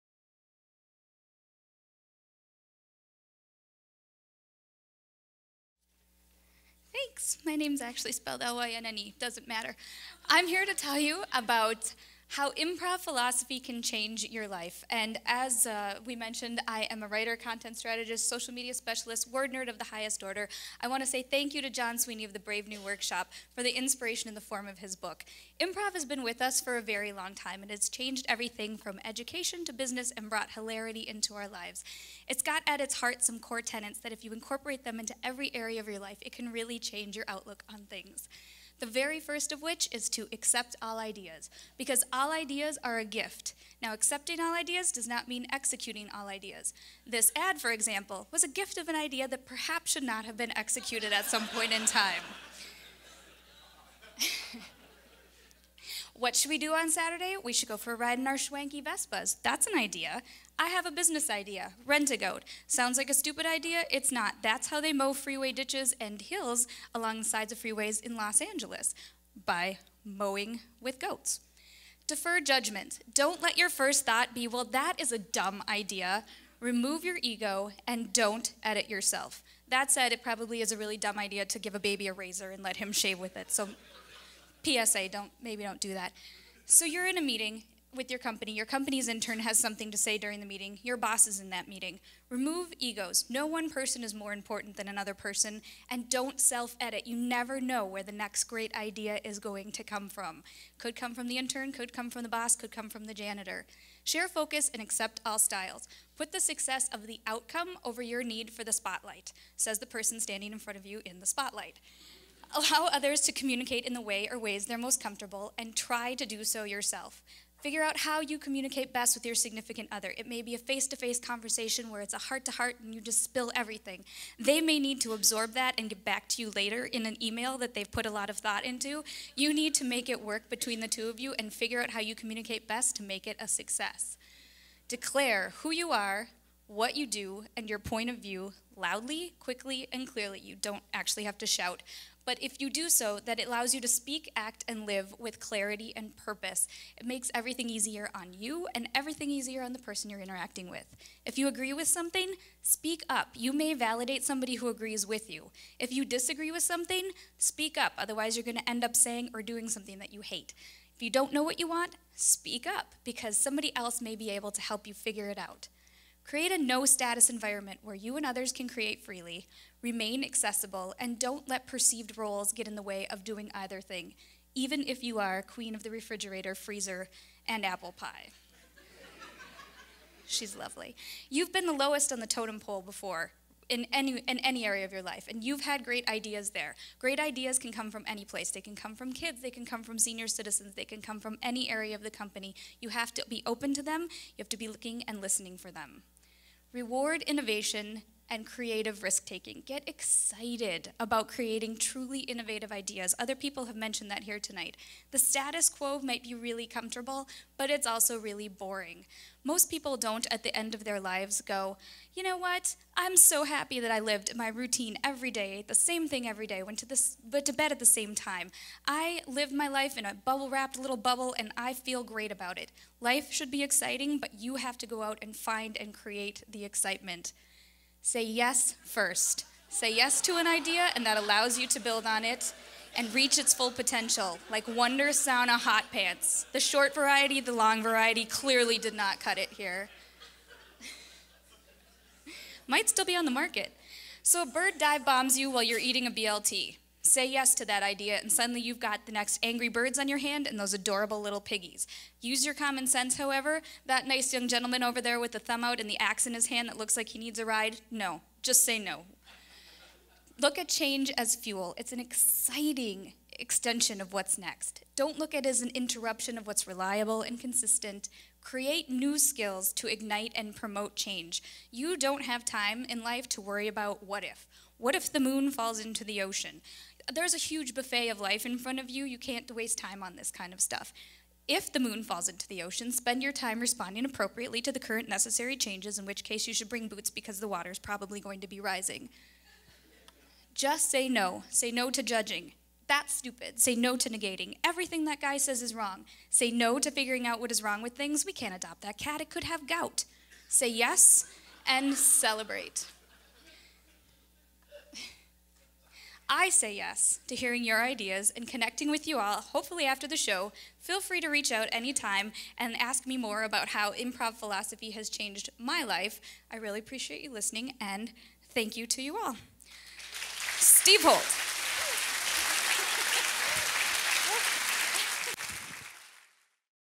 Lynne Morioka - Yes, First! Improv "Philosophy" Will Change Your Life
Lynne Morioka giving her his talk, "Yes, First! How Improv 'Philosophy' Will Change Your Life" for Ignite Minneapolis 5 on May 29, 2013 at the Heights Theater